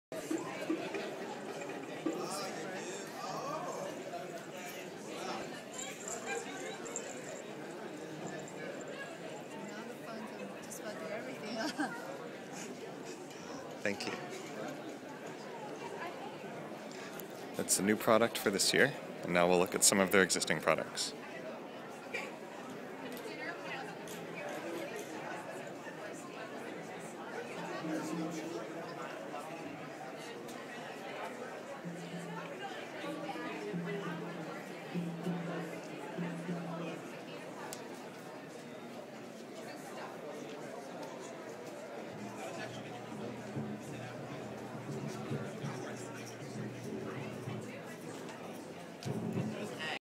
Toy Fair 2012 - Sunny Puppets
Our friends at Sunny Puppets were back again with another fun, playful booth for Toy Fair 2012!